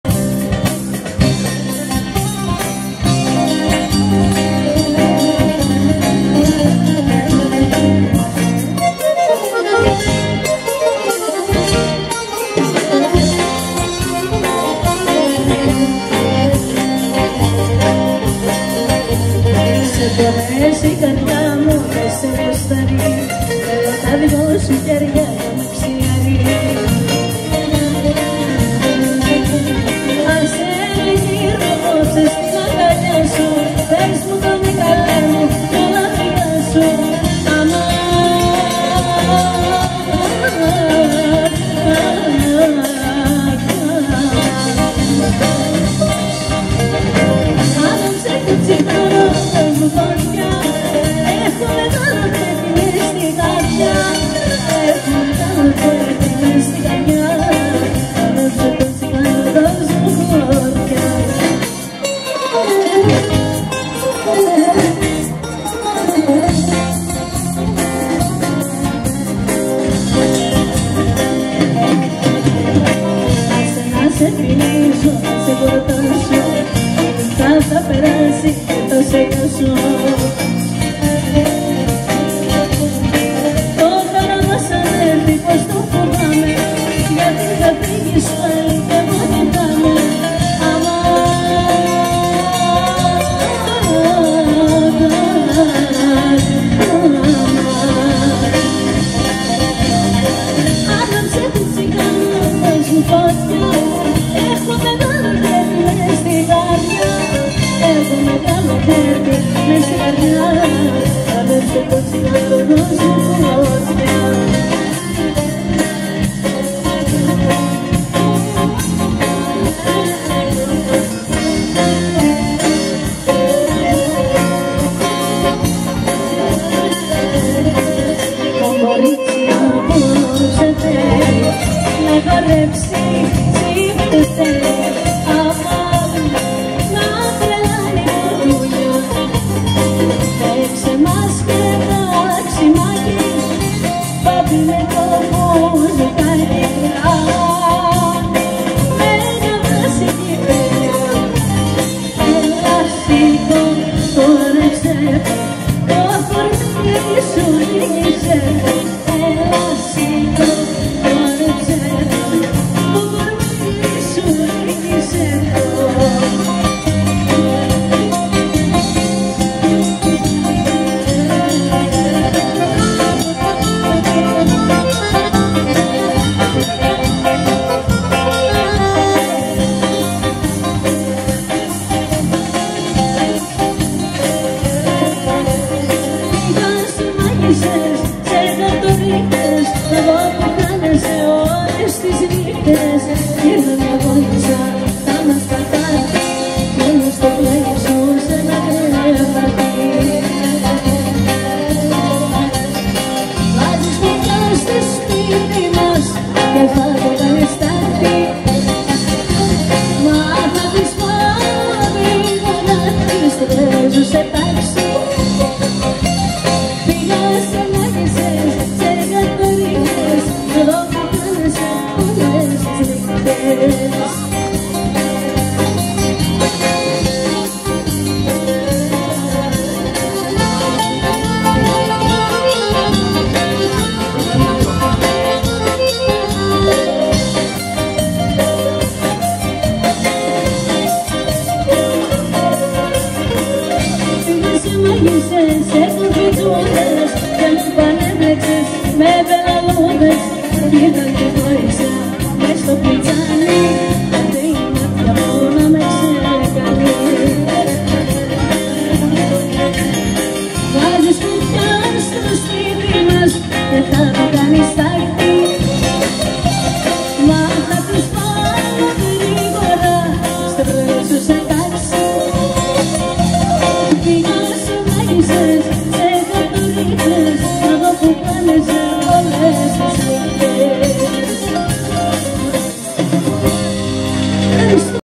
Ραφαέλα Ευαγγελοπούλου στο Μαρκόπουλο, 20-5-2024
Στο Μαρκόπουλο Μεσογαίας στο πανηγύρι του Αγίου Κωνσταντίνου και της Αγίας Ελένης